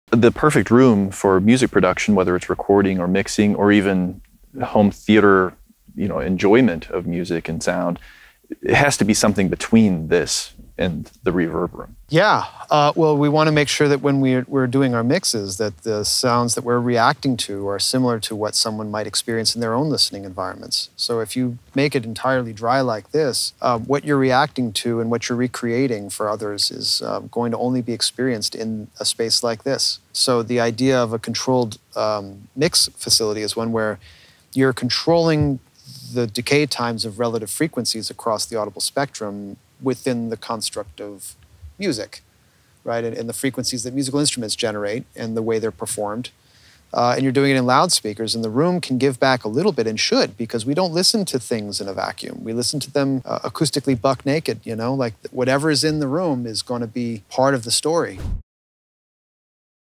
Ideal Room Acoustics for Audio
The importance of balancing dry and reverberant acoustics for audio production.